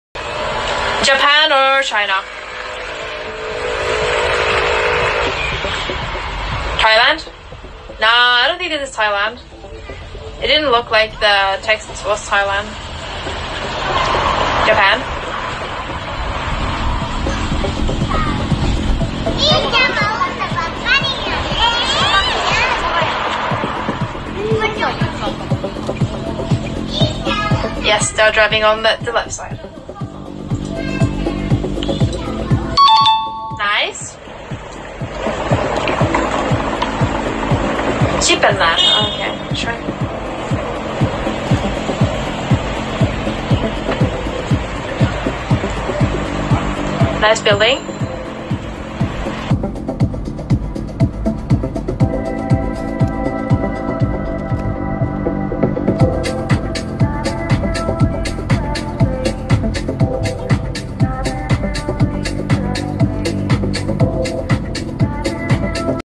Garrison City Of Rawalpindi (Army Capital) IG/Islamabad.Rawalpindi IG/KhyberPashtunKhwa
Veronica Norway Livestreaming .. Murree Road @ Garrison City Of Rawalpindi ( Army Capital )/ Islamabad The Beautiful ( Federal Capital, Founded by Field Marshal Mohammed Ayub Khan )/ Khyber Pashtun Khwa aka Afghania ( KPK )

Instagram.com/JalalFarooqKhanKhattak
Youtube.ru/ChengezKhanKhattak
Instagram.com/Islamabad.Rawalpindi
Vimeo.com/JalalFarooqKhanKhattak
TikTok.com/@ChengezKhanKhattak
TripAdvisor.com/members/ChengezKhanKhattak
Instagram.com/KhyberPashtunKhwa
Instagram.com/ShandoorFestival
Flickr.com/KhanKhattak